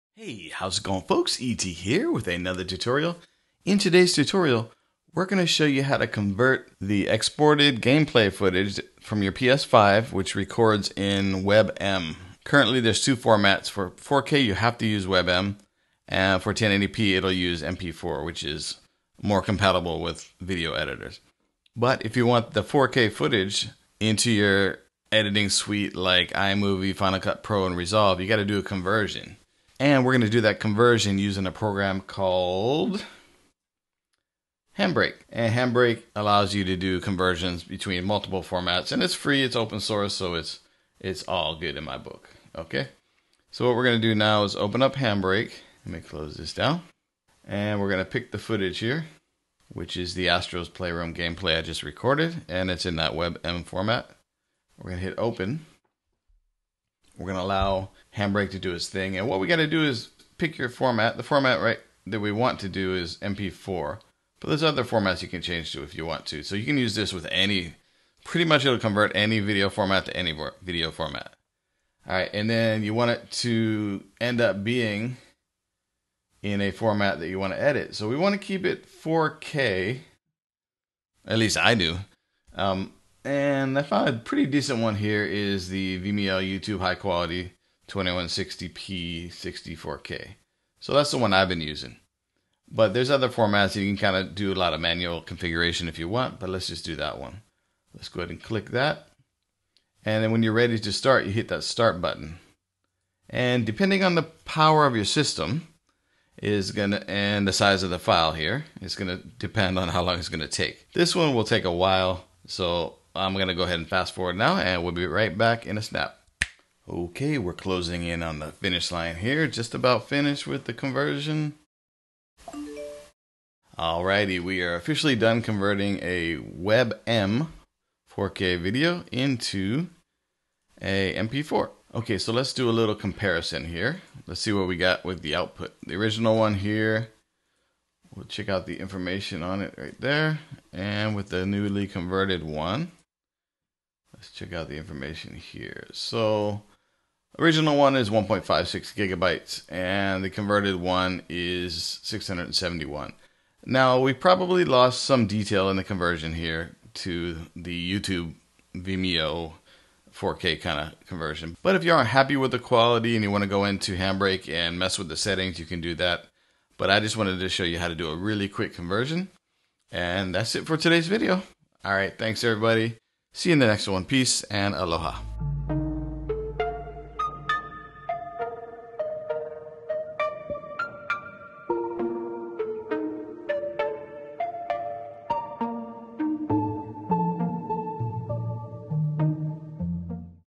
How To Convert PS5 4K WebM to MP4 with HandBrake Tutorial
How to convert Sony Playstation 5 internally captured 4K WebM video to MP4 using HandBrake. This allows you to import the PS5 4K footage into your favorite video editing programs like: iMovie, Final Cut Pro (FCP), DaVinci Resolve, and Adobe Premiere.

If you read this...Comment "Sony Always Wins" in the comments..lol.